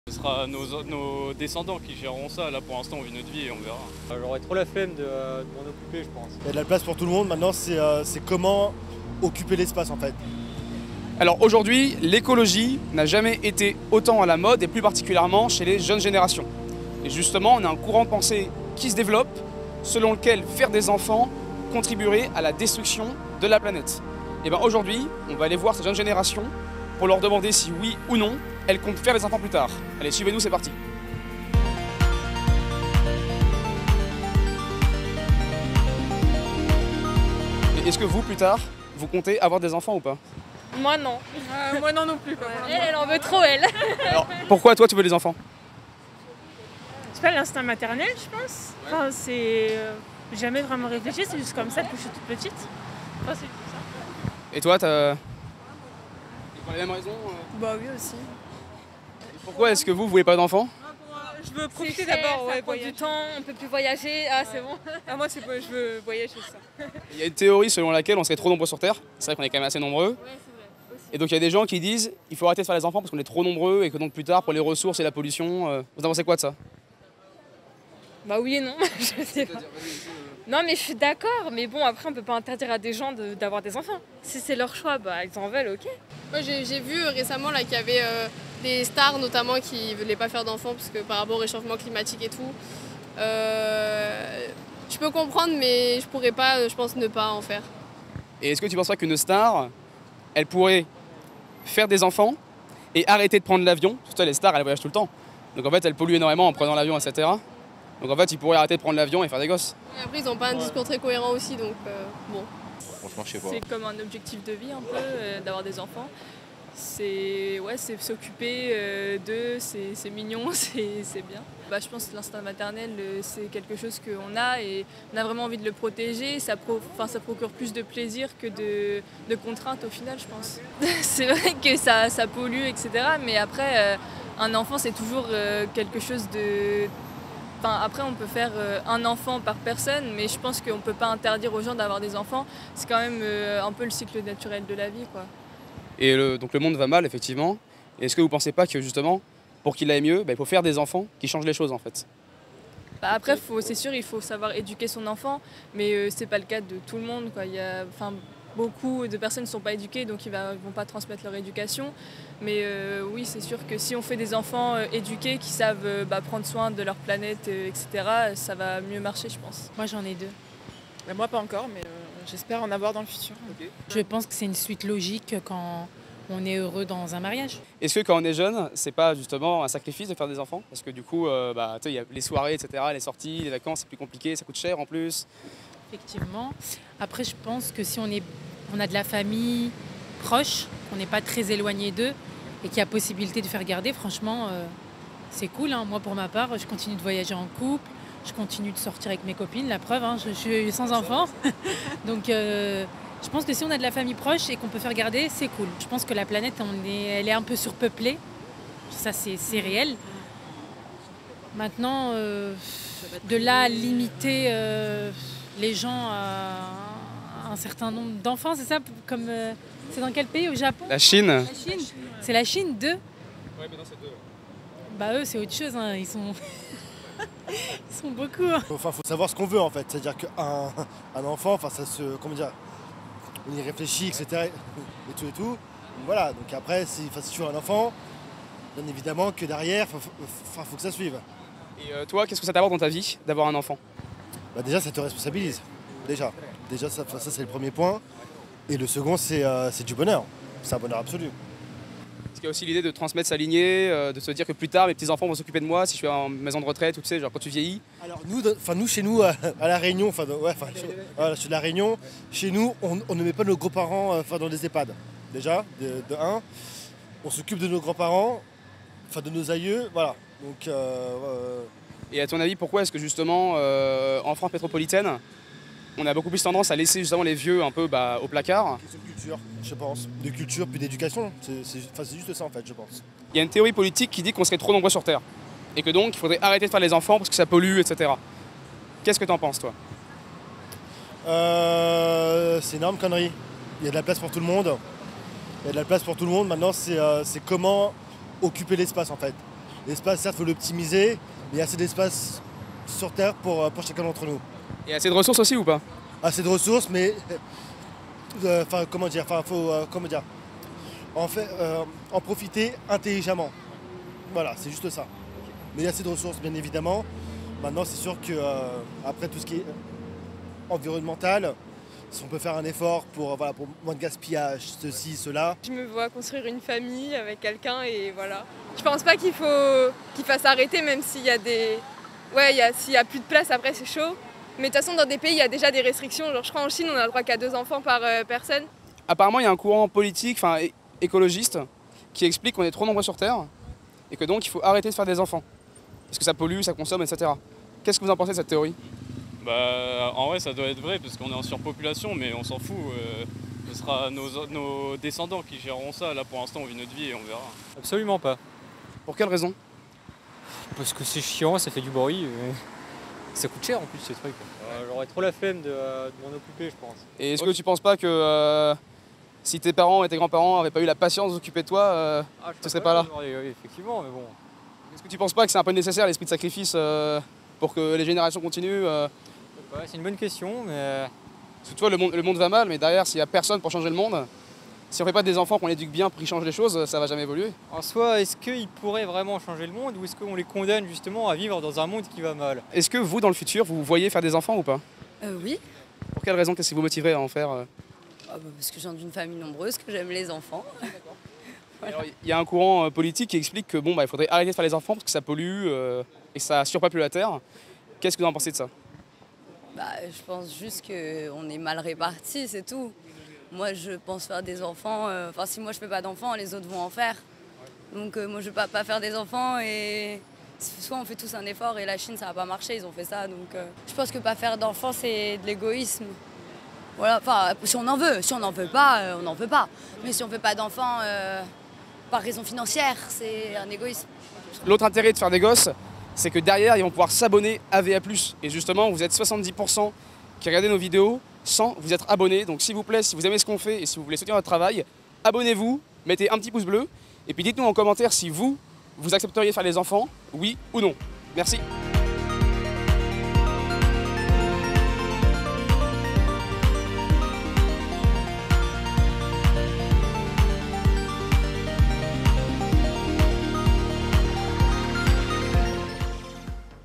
Pourquoi FAIRE (encore) des ENFANTS ? 🍆 💦 [Micro-trottoir]
Selon certains, la Terre serait si surpeuplée que faire des enfants reviendrait à la condamner. Non content de produire un bilan carbone négatif, ces petits êtres demandent engagement et sacrifices. Alors à quoi bon se reproduire à l'heure où les mouvances "Green Inclination No Kid" et "childfree" se font plus en plus influente ?

🔥  ECOUTER aussi VA+ en podcast : ⤵️⤵️